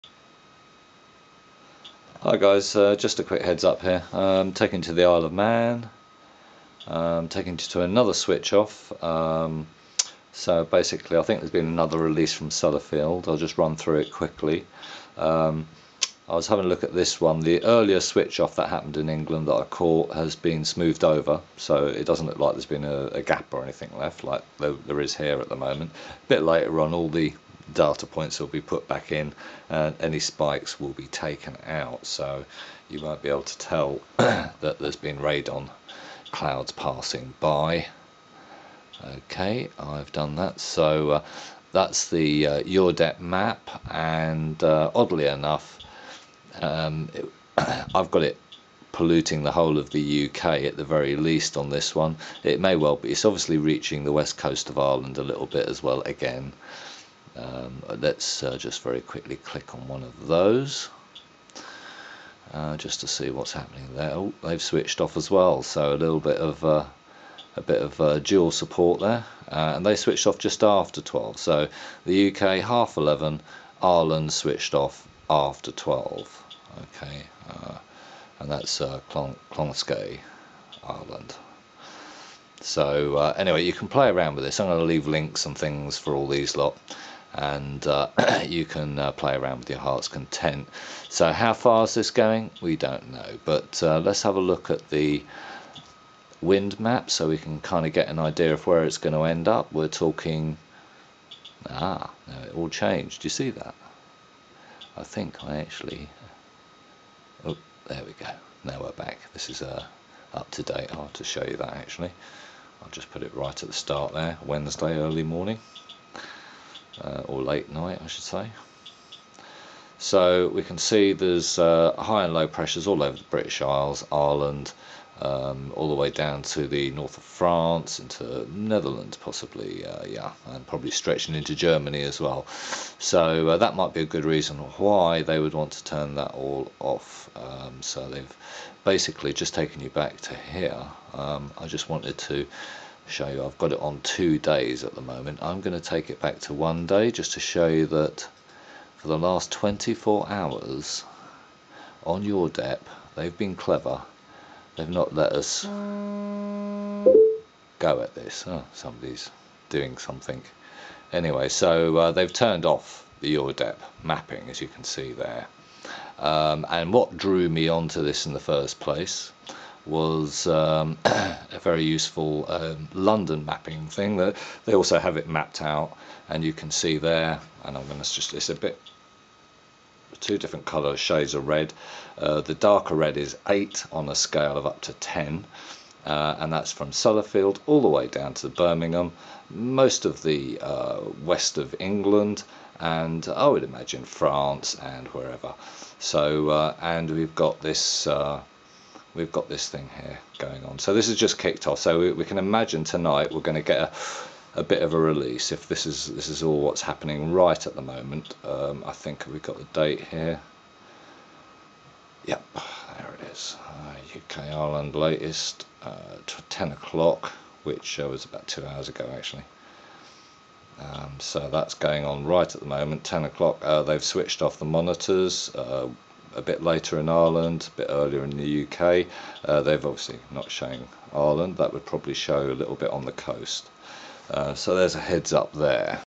Air Quality Warning for NE Europe - Contamination from Sellafield Nuclear site 11 April 2013 Part 1
Dangers of air pollution worse than previously thought, UN health agency warns

8 April 2013 -- The dangers posed by air pollution are far larger than previously thought, the United Nations World Health Organization (WHO) has announced, as it renewed its call for rapid global action in reducing what it described as one of "the greatest hazards to human health.".

."The estimations we have now tell us there are 3.5 million premature deaths every year caused by household air pollution, and 3.3 million death every year caused by outdoor air pollution," Dr. Maria Neira, the WHO's Director of Public Health and Environment, told the CCAC meeting.

Ground-level ozone pollution was estimated to cause an additionally 200,000 premature deaths every year, the agency said in a press release, which notes that "burden of disease" is a calculation based on years of life lost combined with years lived at less than full health.

"Air pollution is becoming one of the biggest health issues we have in front of us at the moment," Dr. Neira said. .

Link to EURDEP radiation monitoring in Europe

Even Moderate Air Pollution Can Raise Stroke Risks

Link to map showing "normal" particulate pollution warnings maps

Feb. 13, 2012 — Air pollution, even at levels generally considered safe by federal regulations, increases the risk of stroke by 34 percent, Beth Israel Deaconess Medical Center researchers have found.